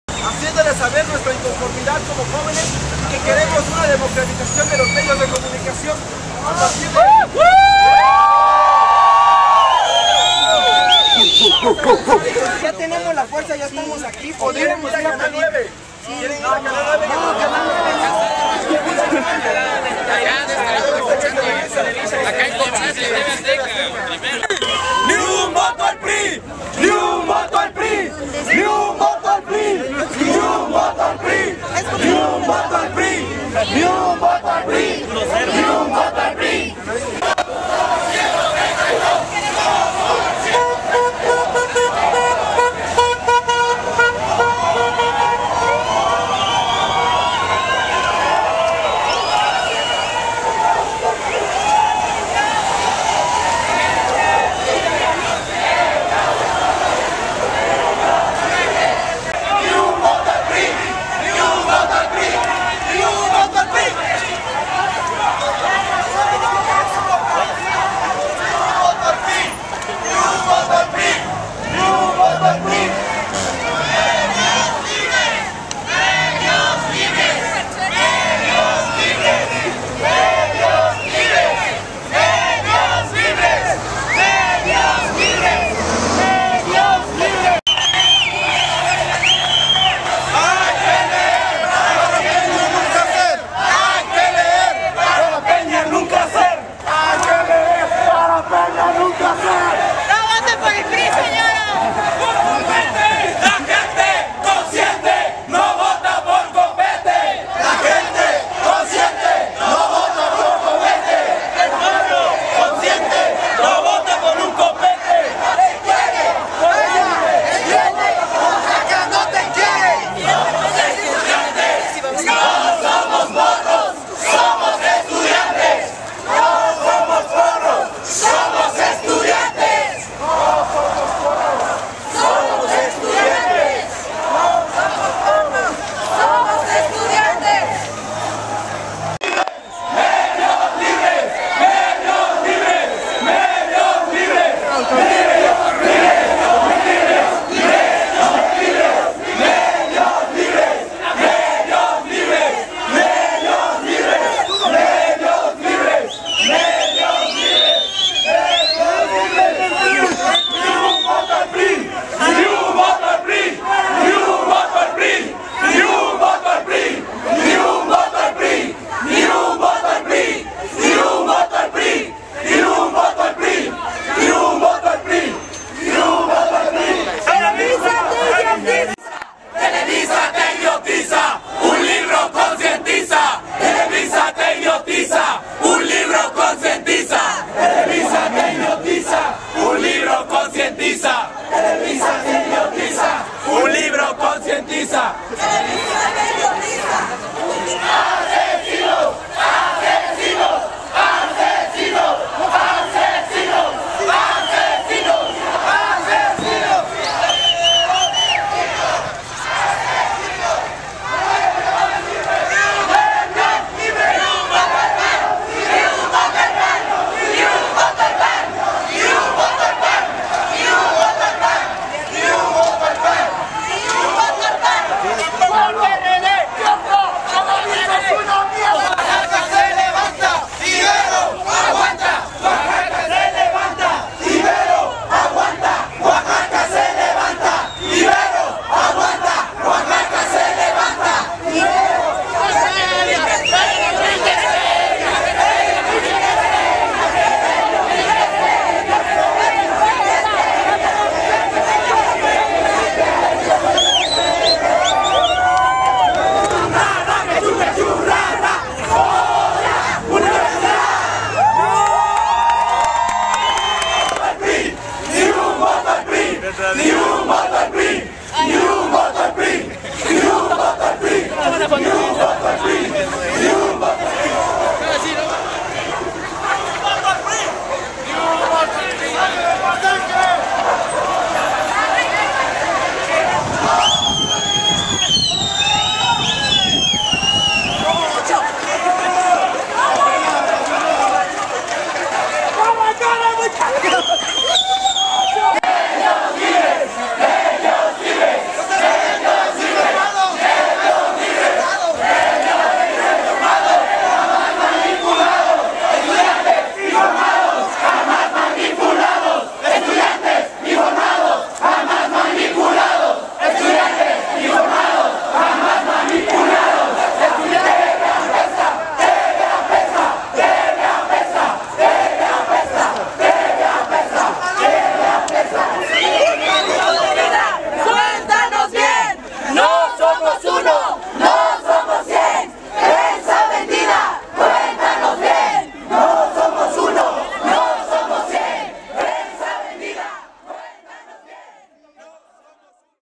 Yo Soy 132 Oaxaca - Marcha 23 de mayo 2012
Protesta ciudadana del Movimiento Yo Soy 132 en Oaxaca, México. 23 de mayo de 2012. Universitari@s unid@s.